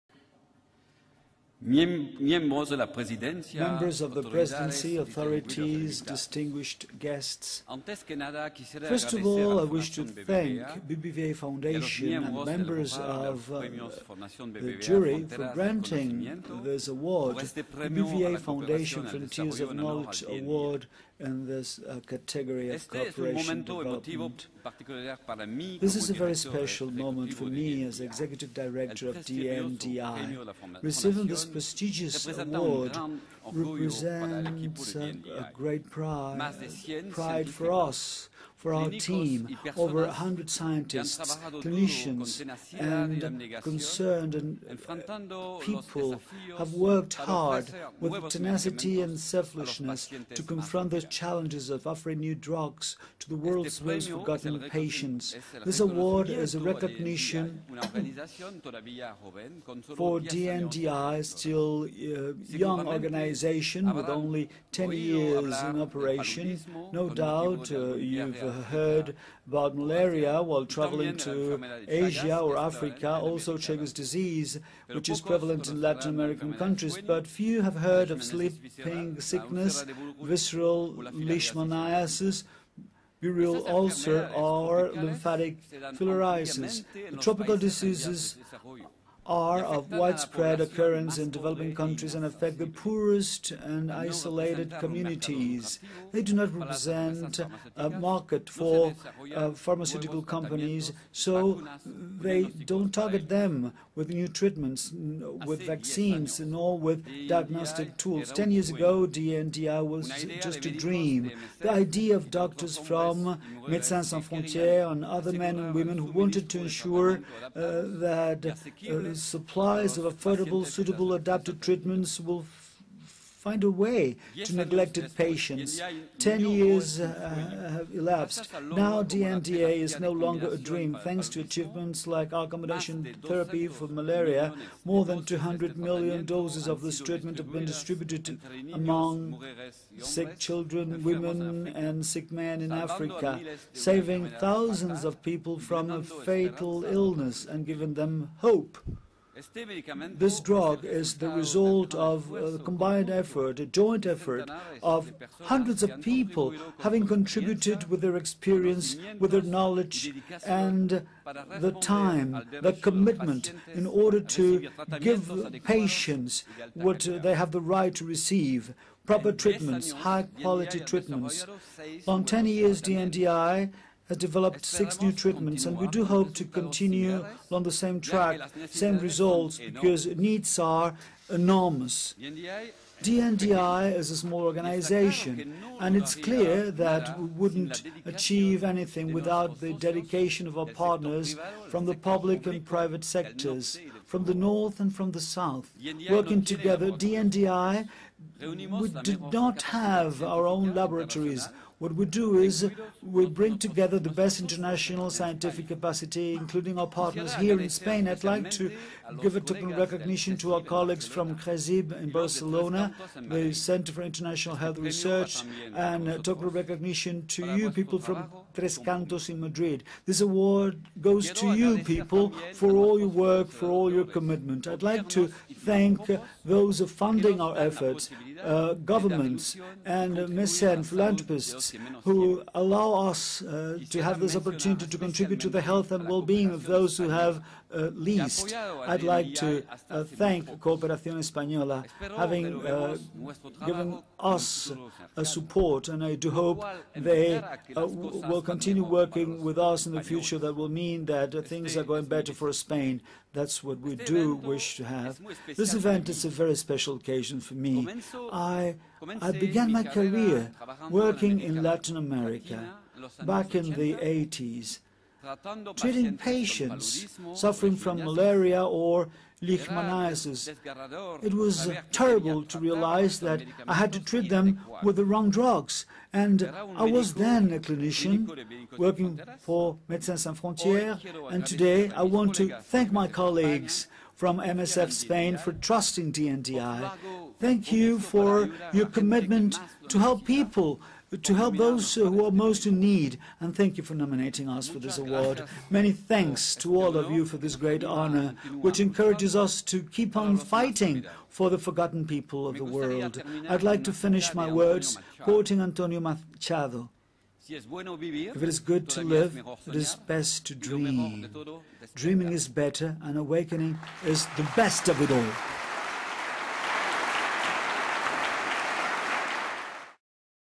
2012 BBVA Foundation Frontiers of Knowledge Awards and Bernard Pécoul
Speech by Bernard Pécoul at the 2012 BBVA Foundation Frontiers of Knowledge Awards